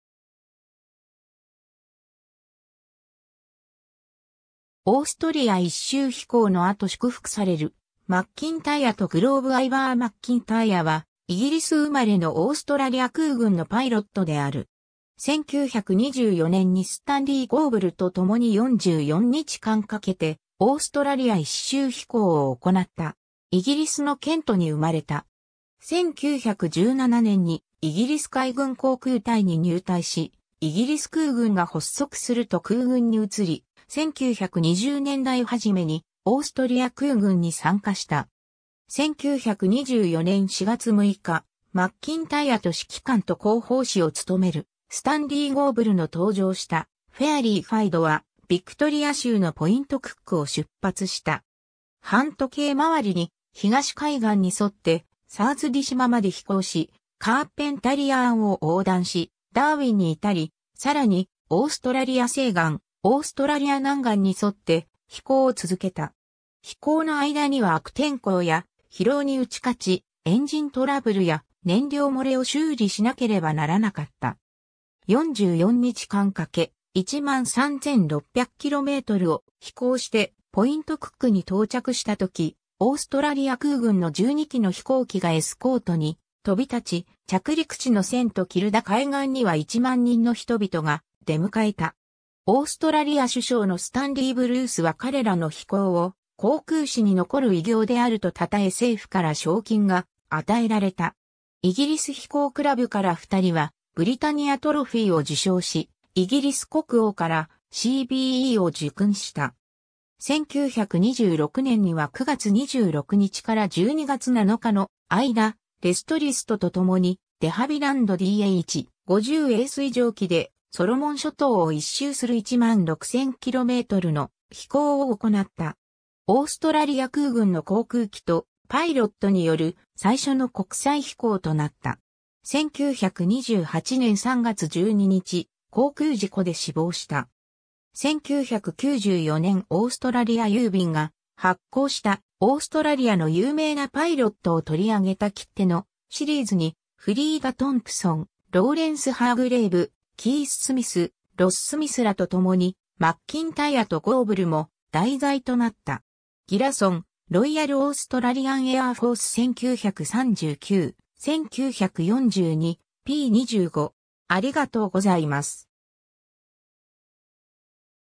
アイヴァー・マッキンタイア
オーストリア一周飛行の後祝福されるマッキンタイア （右）とグローブ アイヴァー・マッキンタイア（Ivor Ewing McIntyre、1899年10月6日 - 1928年3月12日）は、イギリス生まれのオーストラリア空軍のパイロットである。
1924年にスタンリー・ゴーブルとともに44日間かけてオーストラリア一周飛行を行った。
 イギリスのケントに生まれた。
1917年にイギリス海軍航空隊に入隊し、イギリス空軍が発足すると空軍に移り、1920年代初めにオーストリア空軍に参加した。
1924年4月6日、マッキンタイアと指揮官と航法士を務めるスタンリー・ゴーブル（Stanley (Jimmy) Goble）の搭乗したフェアリー　IIIDはビクトリア州のポイント・クックを出発した。
反時計まわりに東海岸に沿ってサーズディ島まで飛行し、カーペンタリア湾を横断し、ダーウィンに至り、さらにオーストラリア西岸、オーストラリア南岸に沿って飛行を続けた。
飛行の間には悪天候や、疲労に打克ち、エンジントラブルや燃料漏れを修理しなければならなかった.。
44日間かけ、13,600kmを飛行して、ポイント・クックに到着した時、オーストラリア空軍の12機の飛行機がエスコートに飛び立ち、着陸地のセント・キルダ海岸には10,000人の人々が出迎えた。
オーストラリア首相のスタンリー・ブルースは彼らの飛行を航空史に残る偉業であるとたたえ政府から賞金が与えられた。
イギリス飛行クラブから2人はブリタニア・トロフィーを受賞し、イギリス国王からCBEを受勲した。
 1926年には9月26日から12月7日の間、レス・トリスト（Les Trist）とともにデハビランド　DH.50A水上機でソロモン諸島を一周する16,000kmの飛行を行った。
オーストラリア空軍の航空機とパイロットによる最初の国際飛行となった。
 1928年3月12日、航空事故で死亡した。
1994年オーストラリア郵便が発行したオーストラリアの有名なパイロットを取り上げた切手のシリーズに、フリーダ・トンプソン、ローレンス・ハーグレーブ、キース・スミス、ロス・スミスらとともに、マッキンタイアとゴーブルも題材となった。
 ^ Gillison, Royal Australian Air Force 1939–1942, p.25